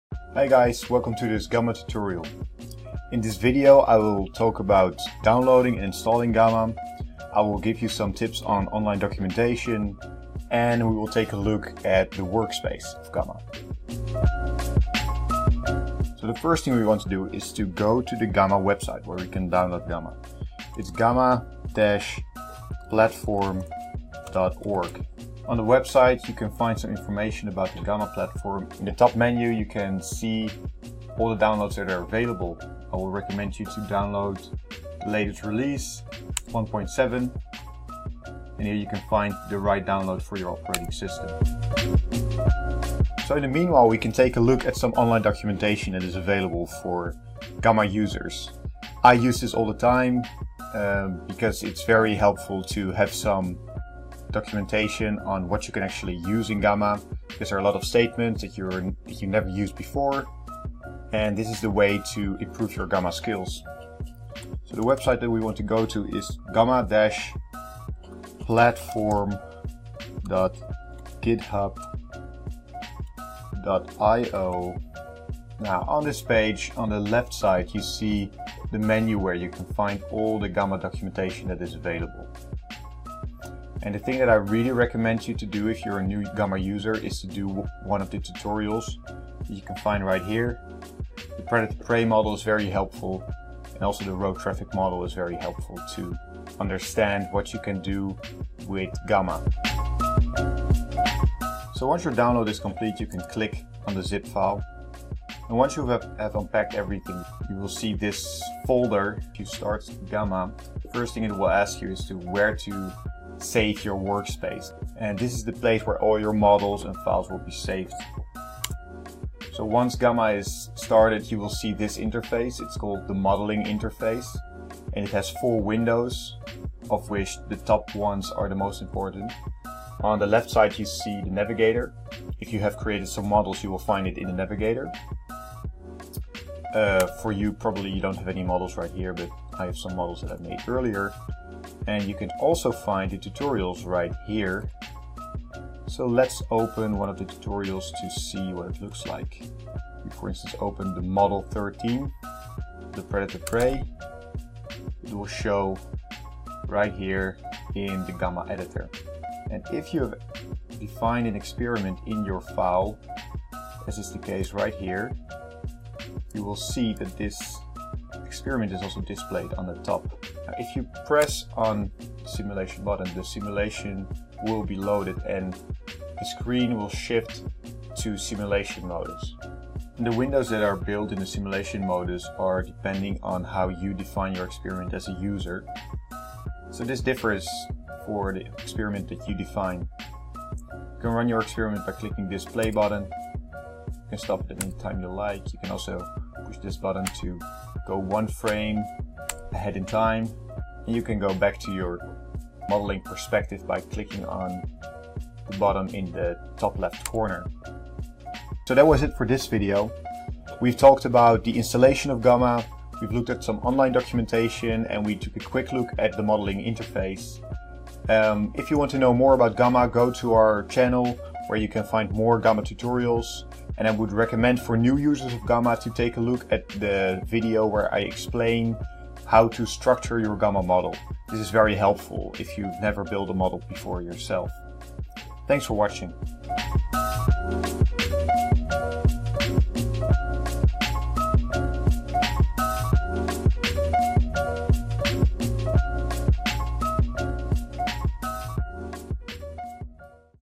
Getting started with GAMA (Installation, documentation and user interface)
This video explains:
(1) How to install GAMA
(2) Where you can find documentation and help for GAMA
(3) How to use the user interface of GAMA

The GAMA Platform is a model development platform to create spatially explicit agent-based simulations. It is used by the Eindhoven University of Technology to develop the SparkCity model. The SparkCity model is capturing the dynamics of the future electric mobility environment, to answer questions about charging infrastructure and EV adoption.